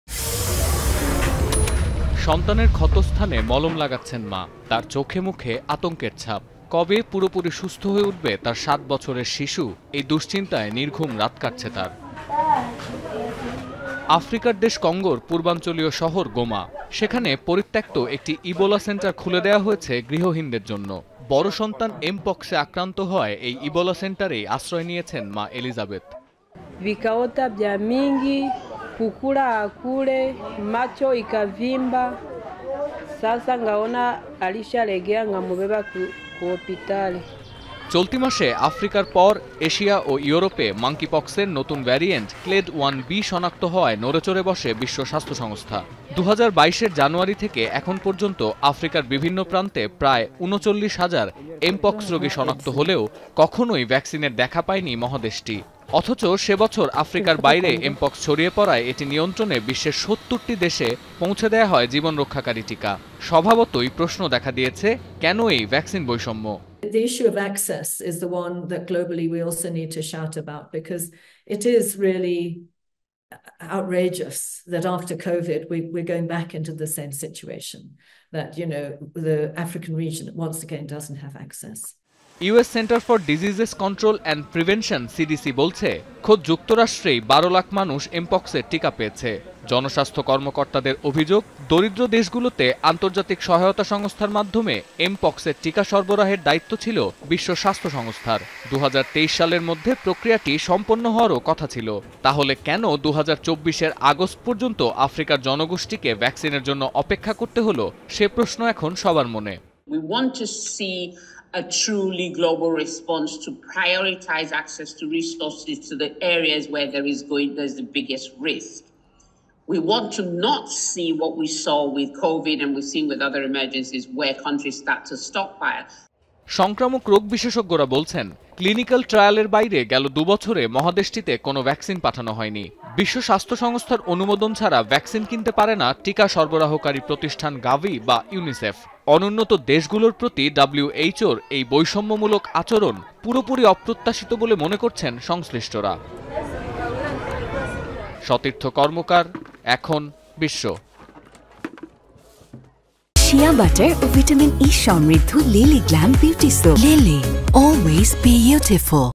এমপক্সের টিকা সরবরাহে বিলম্ব, অভিযোগের তীর বিশ্ব স্বাস্থ্য সংস্থার দিকে! | Monkeypox Vaccine
এমপক্সের টিকা সরবরাহে বিলম্ব, অভিযোগের তীর বিশ্ব স্বাস্থ্য সংস্থার দিকে! | Monkeypox Vaccine | Ekhon TV

Our Office Address:
EKHON TV
City Park Lane
19, Hatkhola Road, Wari
Dhaka-1203
Bangladesh